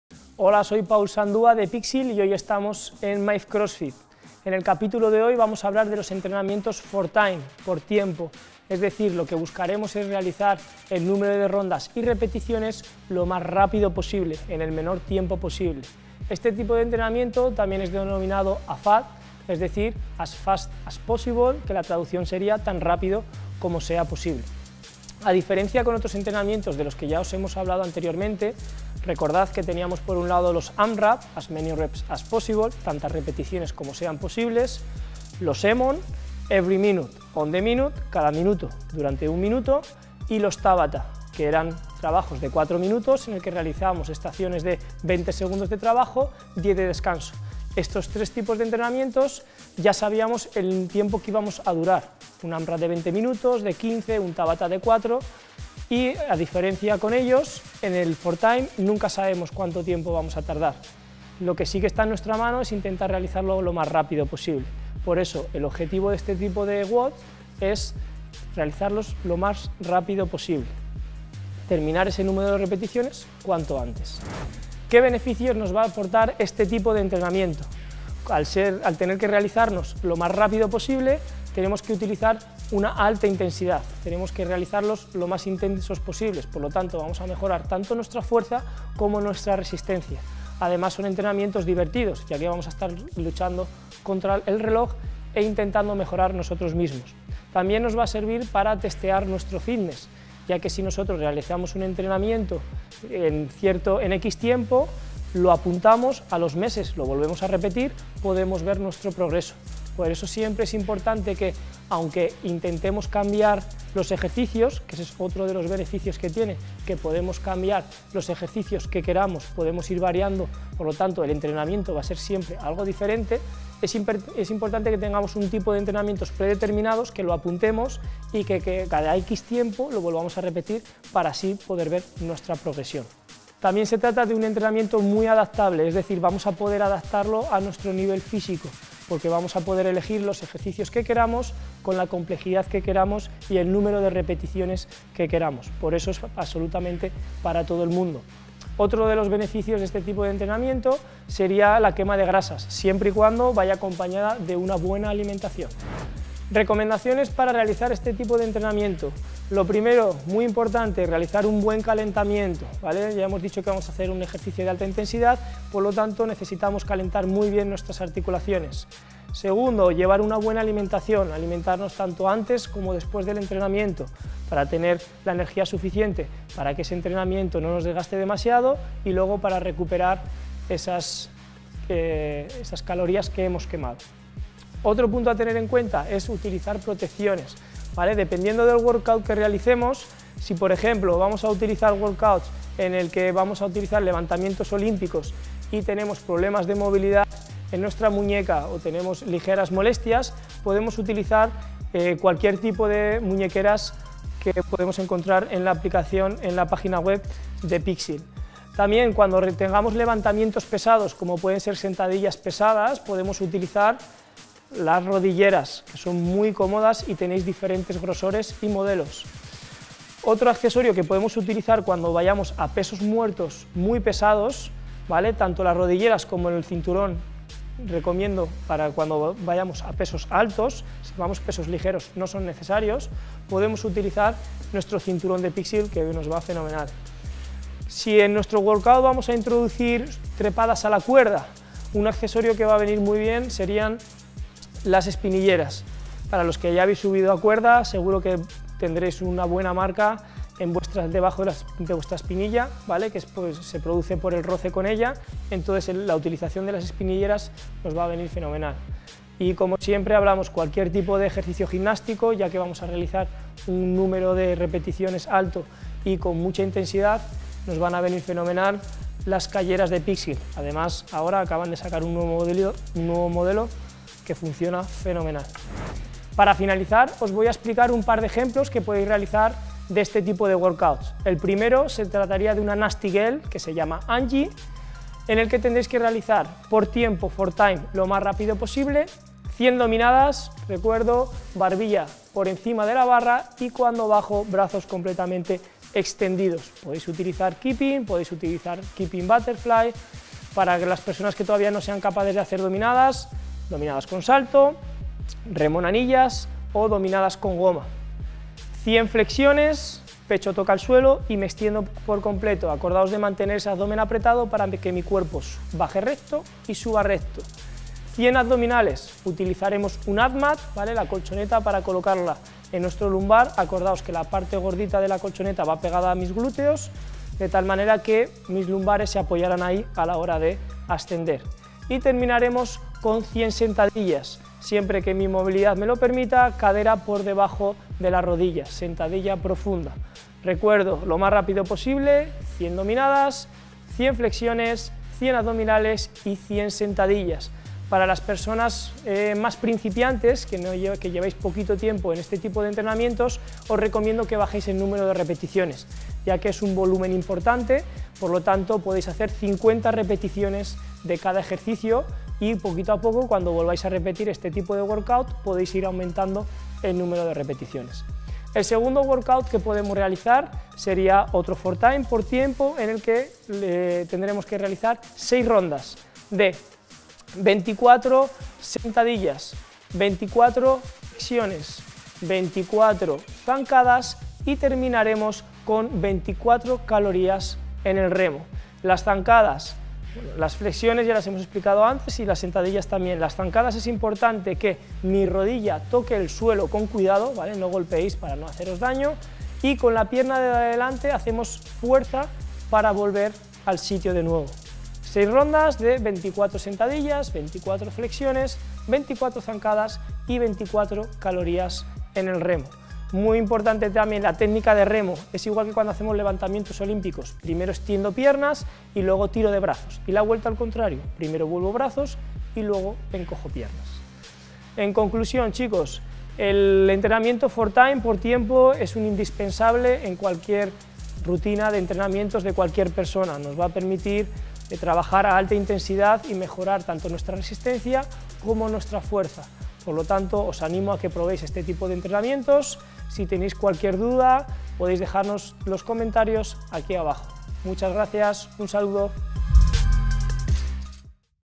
Guía Entrenamientos For Time en Cross Training | Picsil Sport
En crossfit existe un término para cada cosa, lo que te facilita mucho saber a qué te enfrentas cada día en el box, pero seguro que también te traía un poco de cabeza en tus inicios.

Si ese es tu caso en este momento, no te preocupes, dentro de unos minutos sabrás todo sobre el entrenamiento for time.